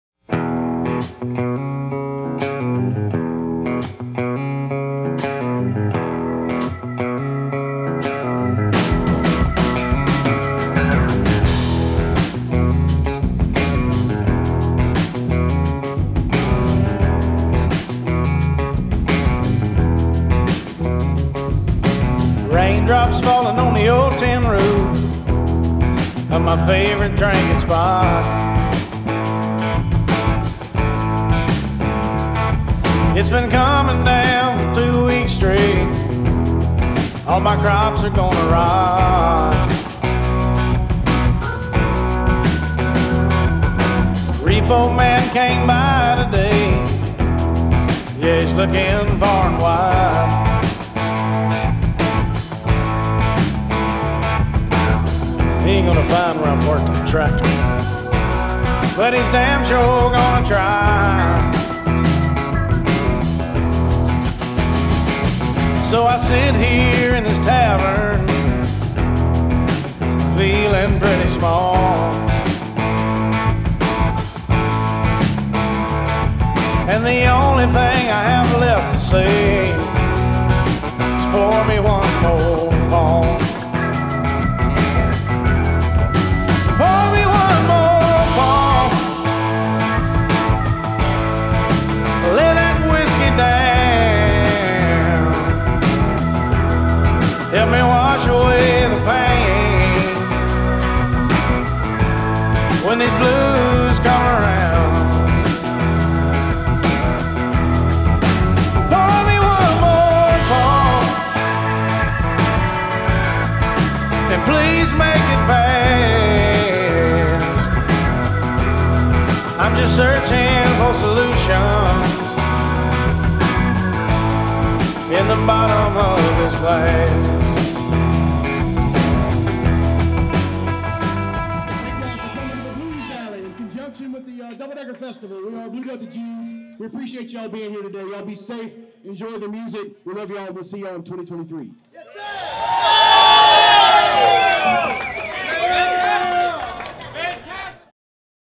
Blues Alley | Double Decker Arts Fest
Blue Delta Jeans puts on the "Blues Alley" stage every year at the Double Decker Arts Festival held in Oxford, MS